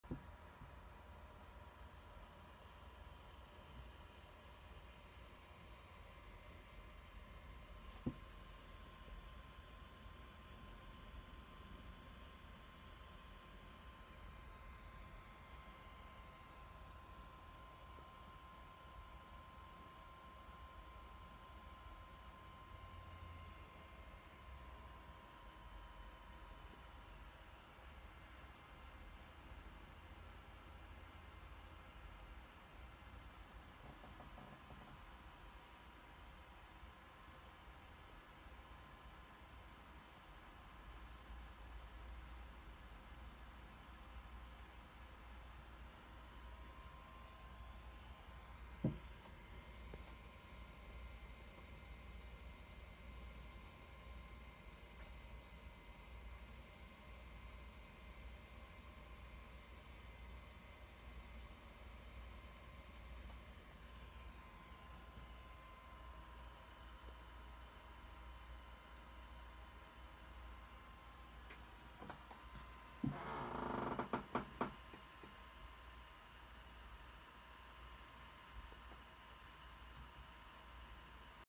Daine
Pointillism: painted with syringes.
Painted by Lucas James Xavier Kolasa